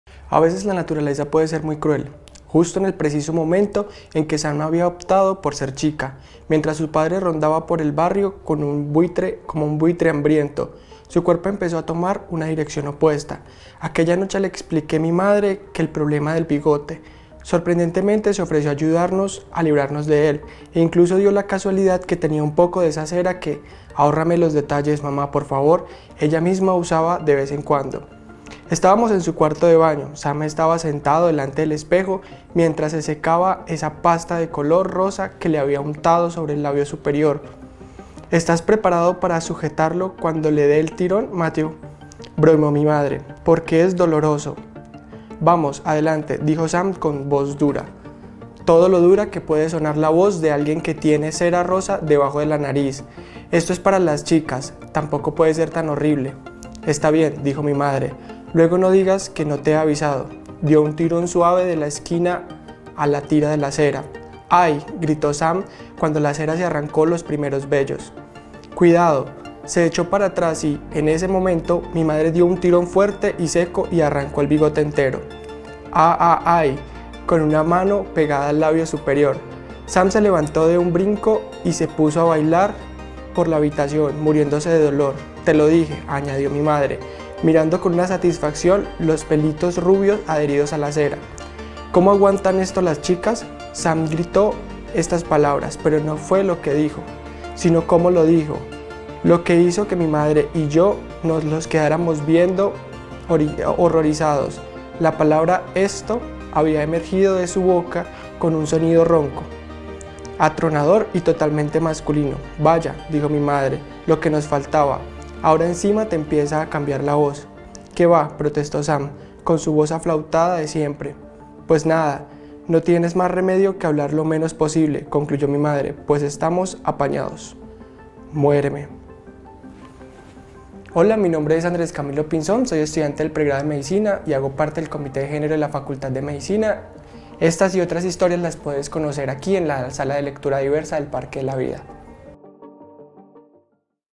Lecturas a Viva voz - De chico a chica", de Terence Blacker
DesdeLaSala traemos la Lectura a Viva Voz de un fragmento del libro "De chico a chica", de Terence Blacker. "Sam López, la nueva alumna del colegio Bradbury Hill, no pasa desapercibida.

Todas las chicas quieren ser como ella y todos los chicos quieren ser su novio. Sin embargo, sólo su primo Matthew y su pandilla saben que, en realidad, Sam es un chico".

Si deseas seguir conociendo más de esta historia, te invitamos a la Sala de Lectura Diversa